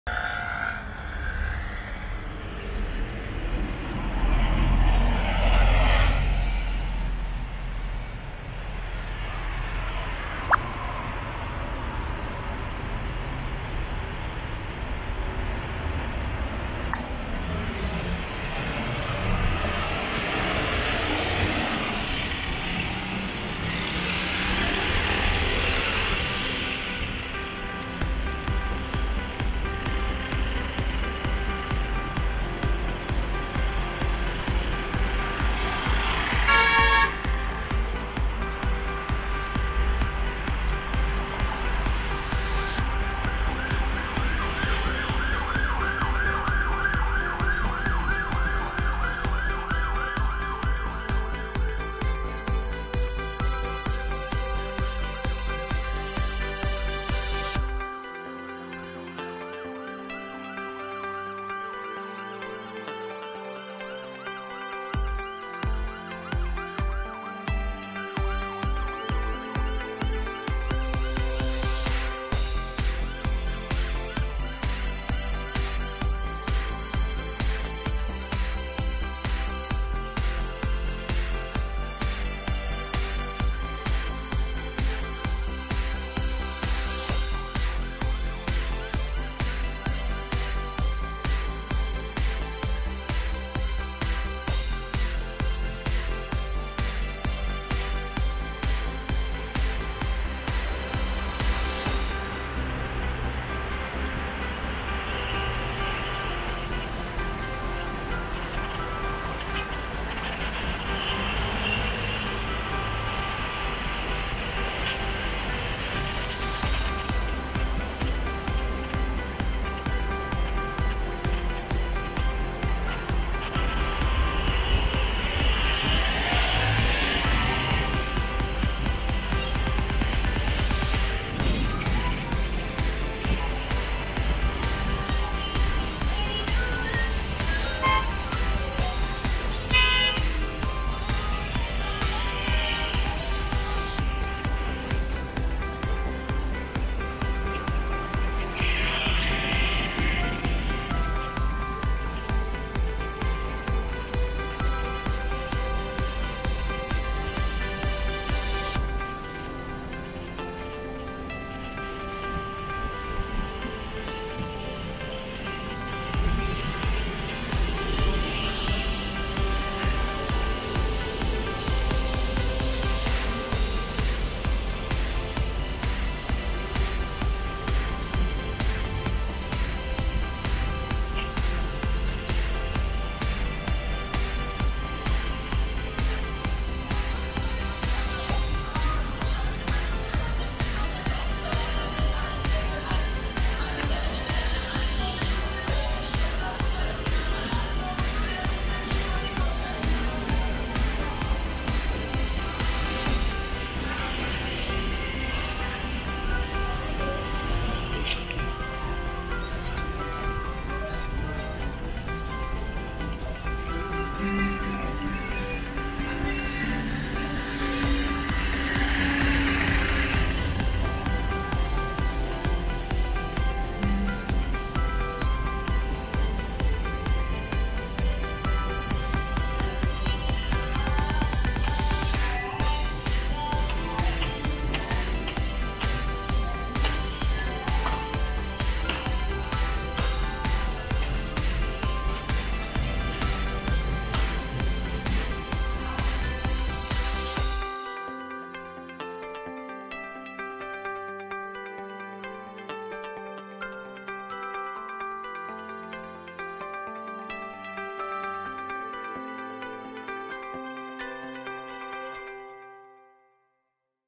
이태원맛집 Korea Seoul noksapyeong station exit 3(녹사평역3번) - Gino's pizza(지노스피자) - Richmond dimsum(리치몬드딤섬)
녹사평역3번출구에서 지노스피자를 거쳐 리치몬드 딤섬으로 가는길 입니다~^^

From Noksapyeong Station Exit 3 to the   Gino's pizza and Richmond dimsum~